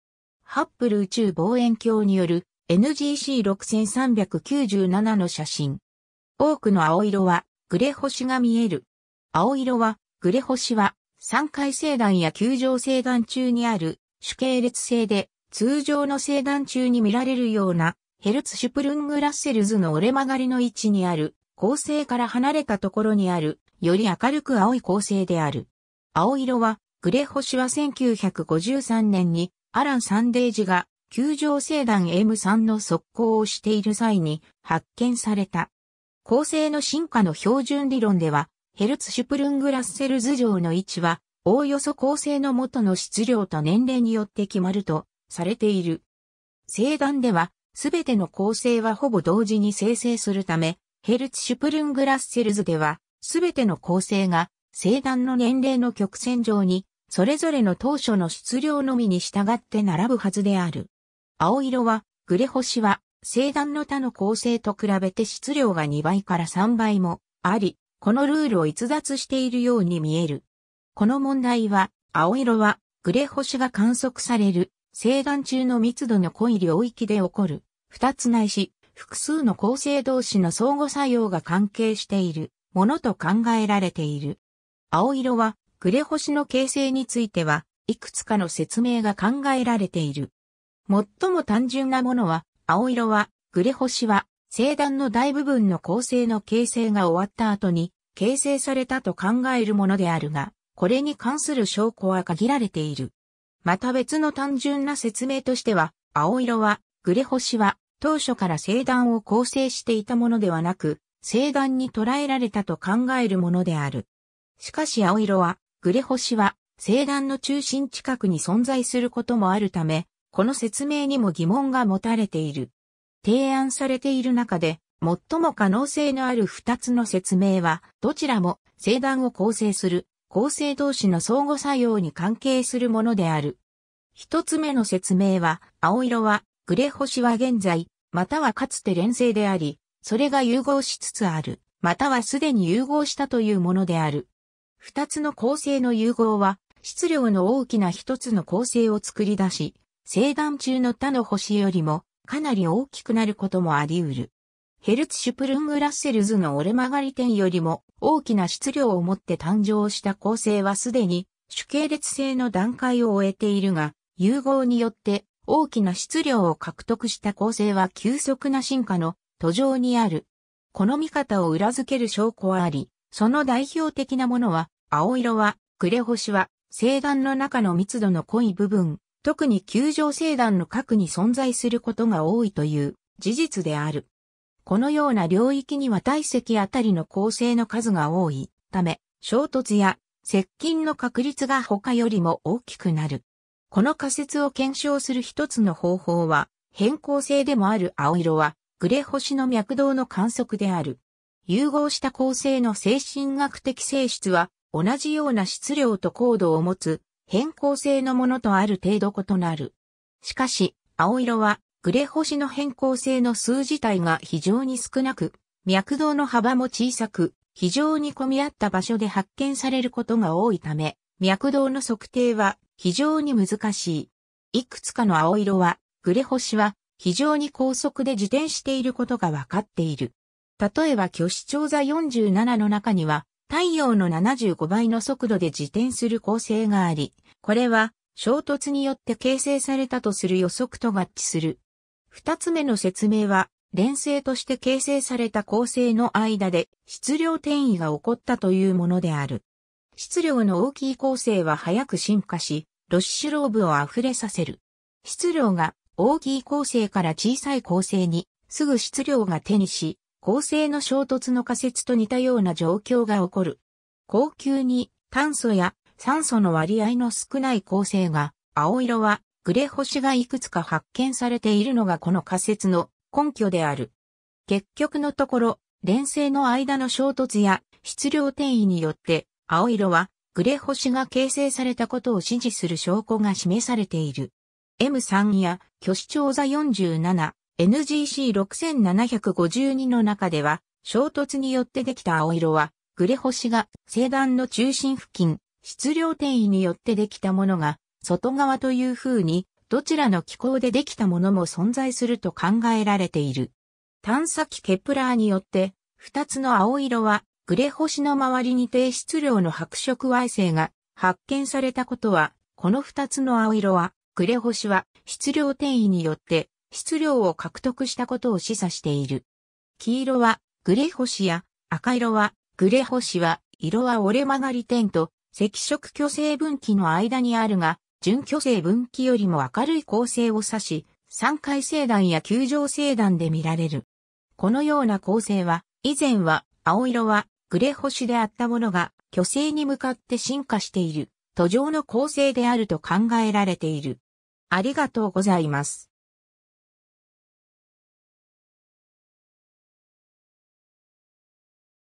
青色はぐれ星
ハッブル宇宙望遠鏡によるNGC 6397の写真。
多くの青色はぐれ星が見える。
 青色はぐれ星（あおいろはぐれぼし、英：blue straggler）は、散開星団や球状星団中にある主系列星で、通常の星団中に見られるようなヘルツシュプルング・ラッセル図の折れ曲がりの位置にある恒星から離れたところにある、より明るく青い恒星である。
青色はぐれ星は1953年にアラン・サンデージが球状星団M3の測光をしている際に発見された。
恒星の進化の標準理論では、ヘルツシュプルング・ラッセル図上の位置は、おおよそ恒星の元の質量と年齢によって決まるとされている。
星団では、全ての恒星はほぼ同時に生成するため、ヘルツシュプルング・ラッセル図では、全ての恒星が星団の年齢の曲線上にそれぞれの当初の質量のみに従って並ぶはずである。
青色はぐれ星は星団の他の恒星と比べて質量が2倍から3倍もあり、このルールを逸脱しているように見える。
この問題は、青色はぐれ星が観測される星団中の密度の濃い領域で起こる、二つないし複数の恒星同士の相互作用が関係しているものと考えられている。
 青色はぐれ星の形成については、いくつかの説明が考えられている。
最も単純なものは、青色はぐれ星は星団の大部分の恒星の形成が終わった後に形成されたと考えるものであるが、これに関する証拠は限られている。
また別の単純な説明としては、青色はぐれ星は当初から星団を構成していたものではなく、星団に捕らえられたと考えるものである。
しかし青色はぐれ星は星団の中心近くに存在することもあるため、この説明にも疑問が持たれている。
 提案されている中で、最も可能性のある2つの説明は、どちらも星団を構成する恒星同士の相互作用に関係するものである。
 1つ目の説明は、青色はぐれ星は現在、またはかつて連星であり、それが融合しつつある、または既に融合したというものである。
2つの恒星の融合は質量の大きな1つの恒星を作り出し、星団中の他の星よりもかなり大きくなることもありうる。
ヘルツシュプルング・ラッセル図の折れ曲がり点よりも大きな質量を持って誕生した恒星は既に主系列星の段階を終えているが、融合によって大きな質量を獲得した恒星は急速な進化の途上にある。
この見方を裏付ける証拠はあり、その代表的なものは、青色はぐれ星は星団の中の密度の濃い部分、特に球状星団の核に存在することが多いという事実である。
このような領域には体積当たりの恒星の数が多いため、衝突や接近の確率が他よりも大きくなる。
 この仮説を検証する1つの方法は、変光星でもある青色はぐれ星の脈動の観測である。
融合した恒星の星震学的性質は、同じような質量と光度を持つ変光星のものとある程度異なる。
しかし、青色はぐれ星の変光星の数自体が非常に少なく、脈動の幅も小さく、非常に混み合った場所で発見されることが多いため、脈動の測定は非常に難しい。
いくつかの青色はぐれ星は非常に高速で自転していることが分かっている。
例えばきょしちょう座47の中には、太陽の75倍の速度で自転する恒星があり、これは衝突によって形成されたとする予測と合致する。
 2つ目の説明は、連星として形成された恒星の間で質量転移が起こったというものである。
質量の大きい恒星は速く進化し、ロッシュ・ローブを溢れさせる。
質量が大きい恒星から小さい恒星にすぐ質量が転移し、恒星の衝突の仮説と似たような状況が起こる。
光球に炭素や酸素の割合の少ない恒星が青色はぐれ星がいくつか発見されているのがこの仮説の根拠である。
 結局のところ、連星の間の衝突や質量転移によって青色はぐれ星が形成されたことを支持する証拠が示されている。
M3やきょしちょう座47、NGC 6752の中では、衝突によってできた青色はぐれ星が星団の中心付近、質量転移によってできたものが外側という風に、どちらの機構でできたものも存在すると考えられている。
探査機ケプラーによって、2つの青色はぐれ星の周りに低質量の白色矮星が発見されたことは、この2つの青色はぐれ星は質量転移によって質量を獲得したことを示唆している。
 「黄色はぐれ星」や「赤色はぐれ星」は、色は折れ曲がり点と赤色巨星分岐の間にあるが、準巨星分岐よりも明るい恒星を指し、散開星団や球状星団で見られる。
このような恒星は、以前は青色はぐれ星であったものが巨星に向かって進化している途上の恒星であると考えられている。